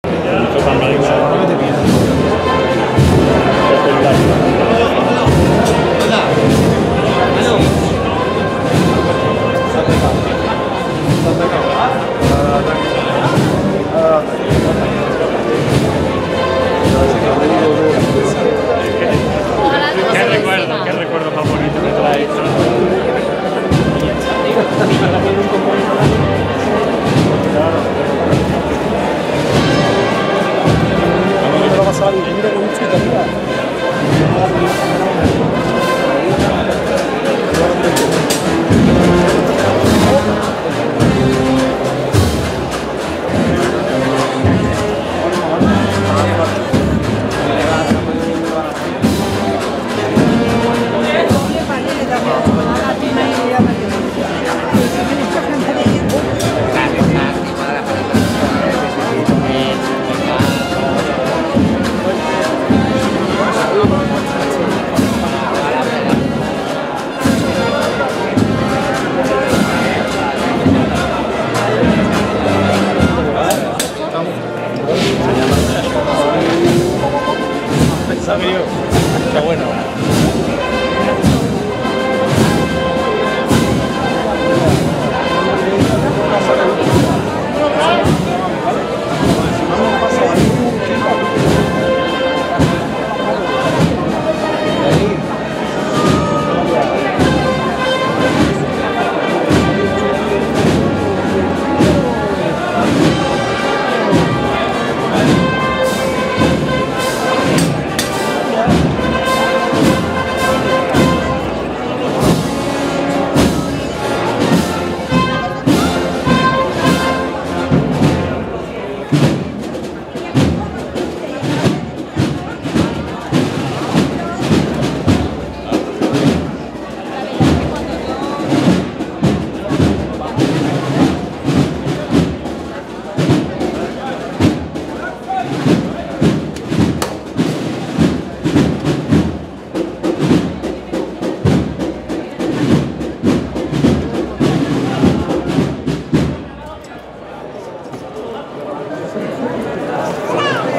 Divina Enfermera por Doiz 2015
La Esperanza Divina Enfermera transcurriendo por la Estrechez de la Calle Daoiz a sones de la BM:"Cigarreras"